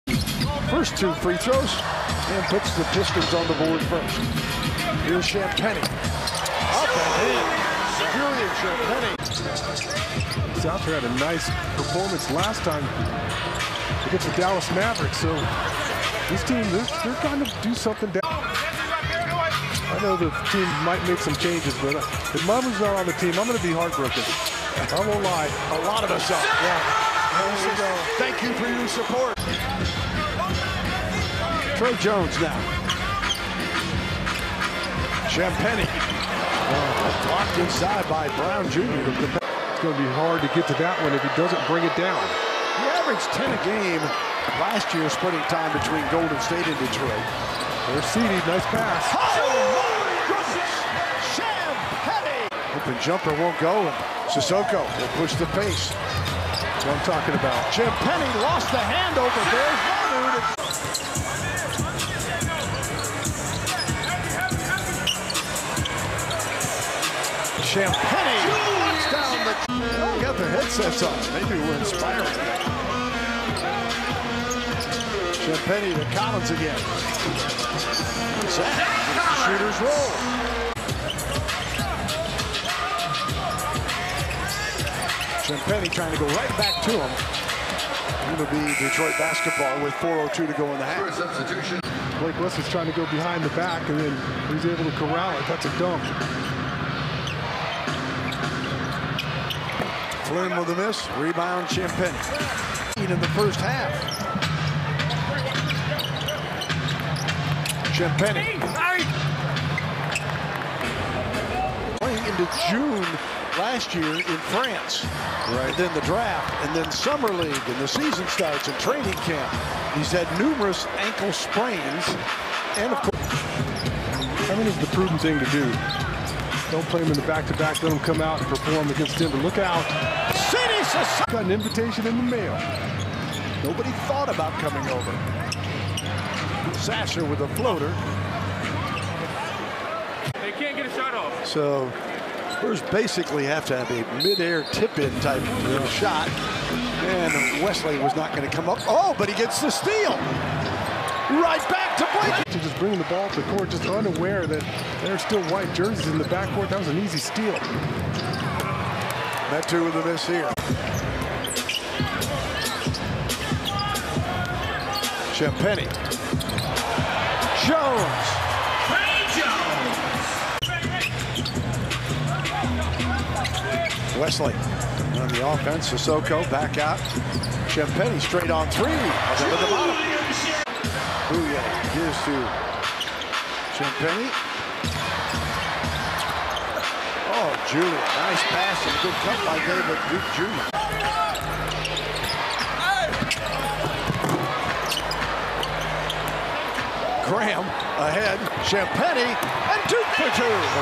Julian Champagnie Player Full Highlights VS PISTONS NBA Regular Season Game 14-04-2024
Check out the latest highlights of Julian Champagnie from the NBA Regular Season between SPURS vs PISTONS. Watch as Julian Champagnie was preforming on the court with his field goal attempts, rebounds, turnovers, steals, assists, and blocks. This video includes all of Julian  gameplays possessions from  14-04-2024.